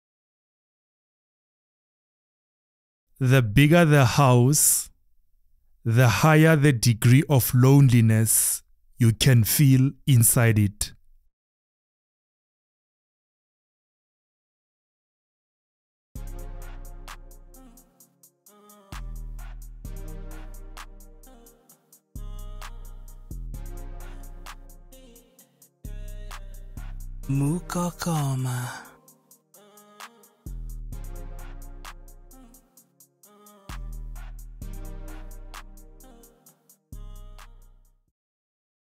APHORISM 880 • DO YOU DISAGREE?👂🏾
Get TWICE as many UNPUBLISHED aphorisms (“quotes”) of mine (for FREE, via my online School for Students of Life) than I share

“The bigger the house, the higher the degree of loneliness you can feel inside it.” — @Mokokoma Mokhonoana